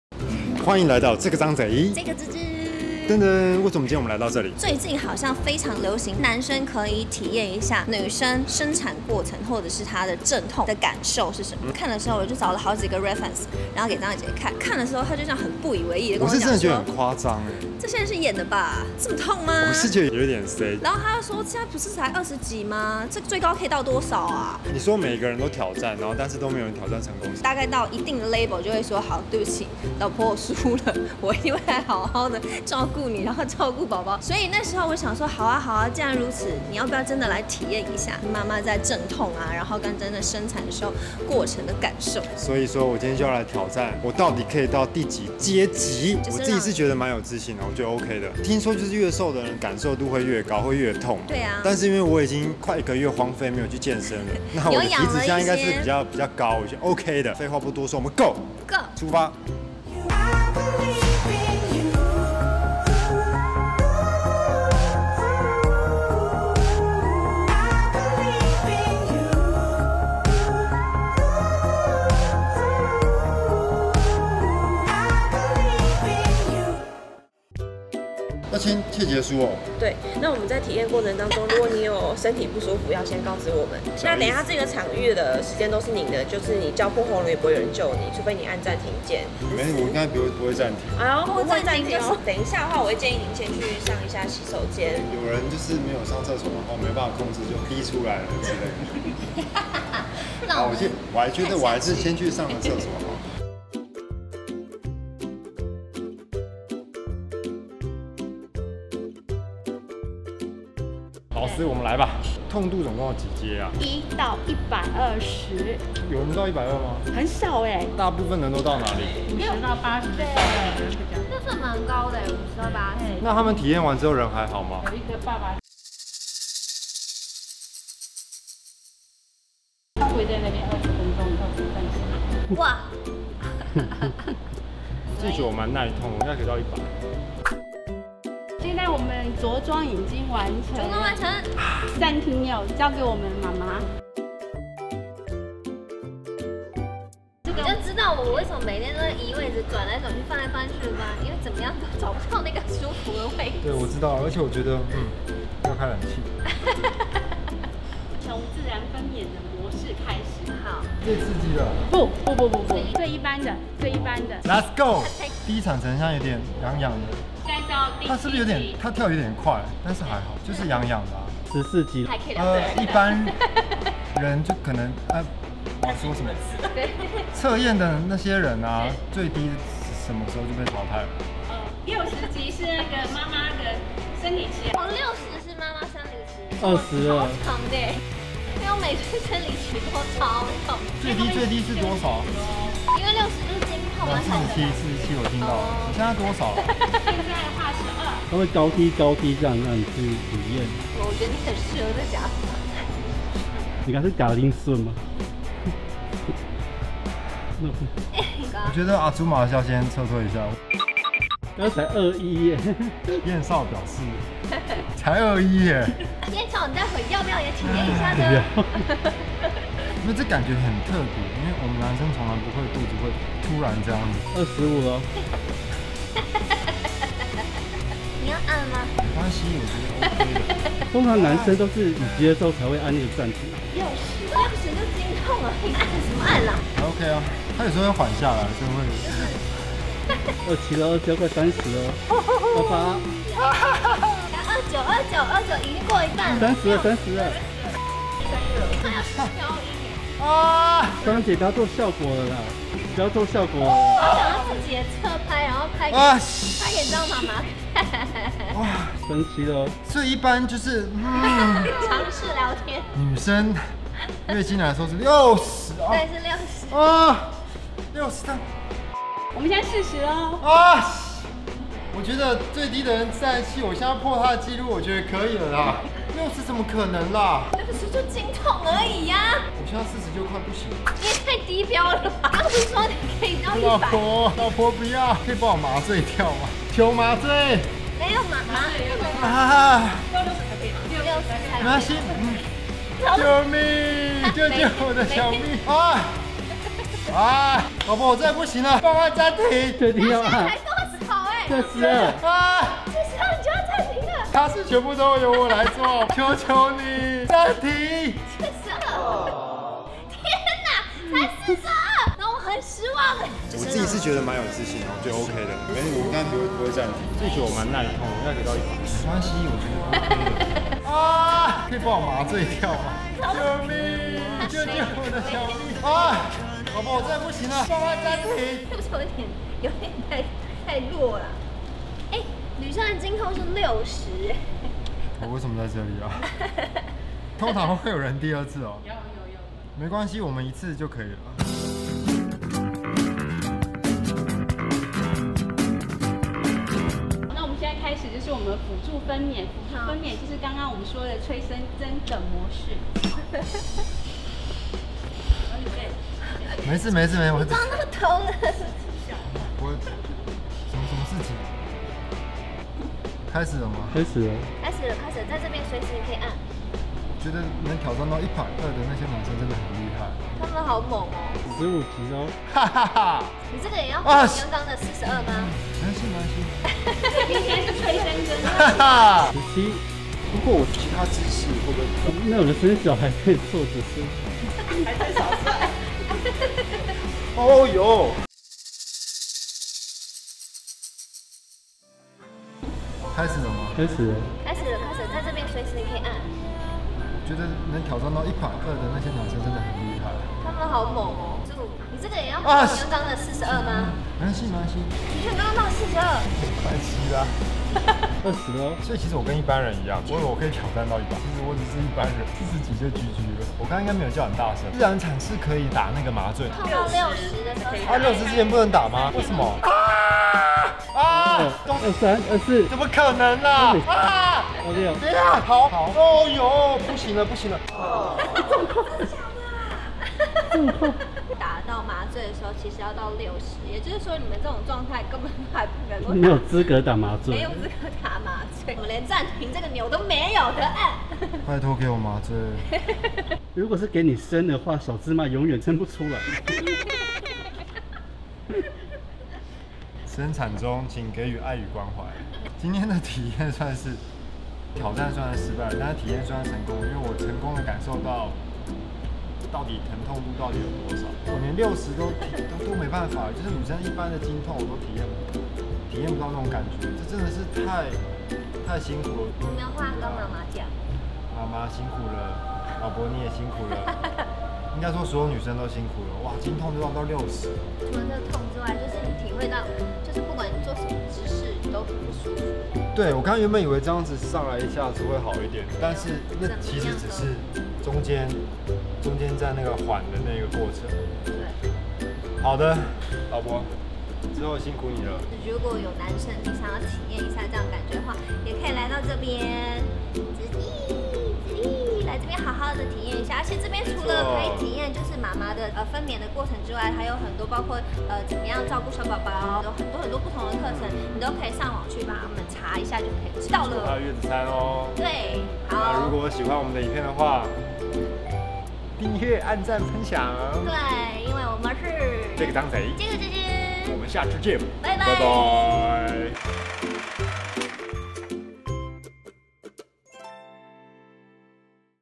張捷體驗生產過程，其實是芝芝的布局？！
讓先生體驗媽媽的生產過程立意雖好，但會不會是媽媽界的一場深遠的佈局？！

▶YOUTUBE頻道 搜尋 聯絡簿拿來我簽!